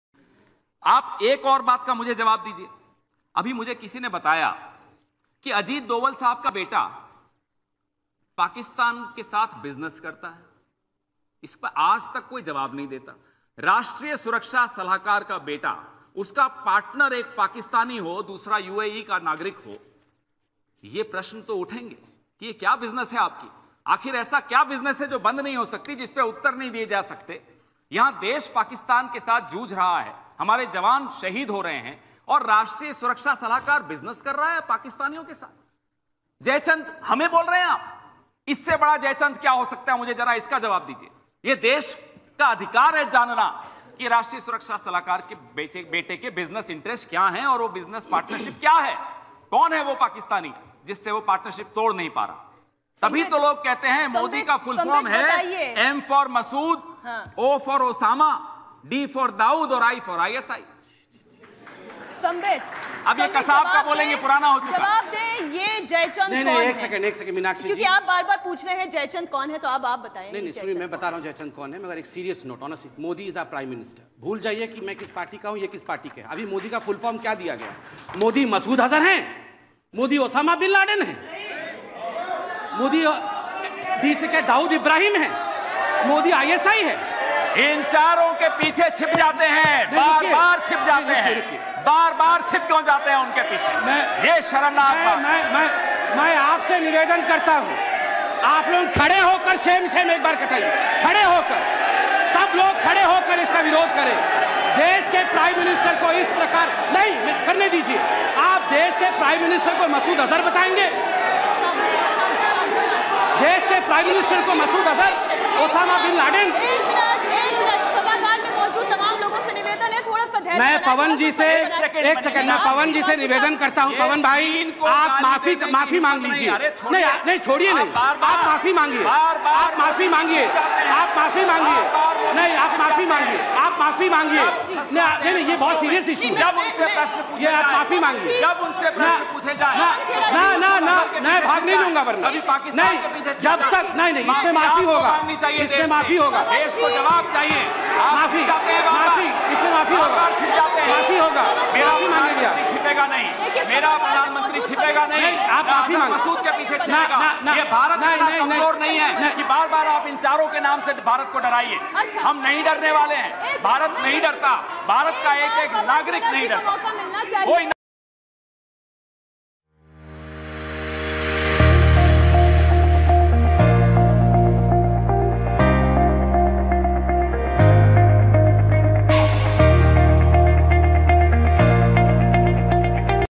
Must Watch | Pawan Khera Controversial Statement on PM Modi | Bol India bol
जरुर देखें | ये क्या कह गए पवन खेड़ा | बता डाला मोदी का फुल फॉर्म -- | गुस्सा लाल बबूला हुए बीजेपी के प्रवक्ता | बोल इंडिया बोल